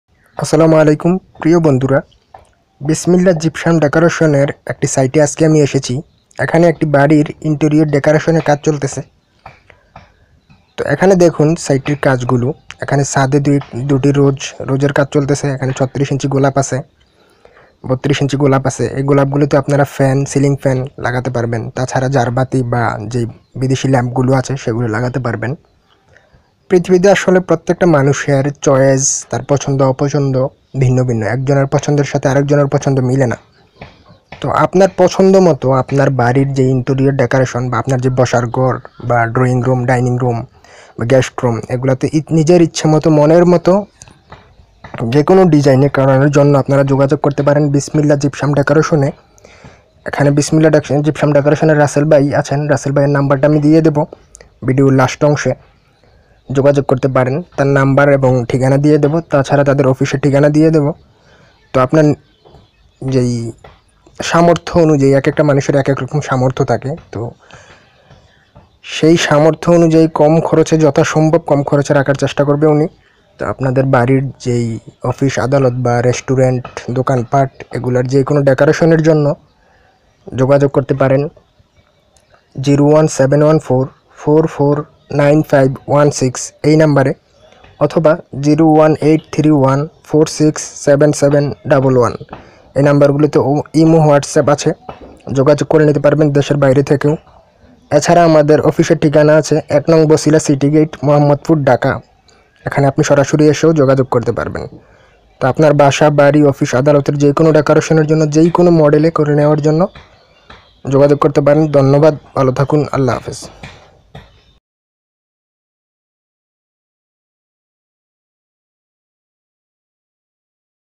ইন্টেরিয়র ডেকারেশন || আপনার ড্রয়িং, ডাইনিং ও বেডরুম সাজবে মনের মত করে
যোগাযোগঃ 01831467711
imo, whatsapp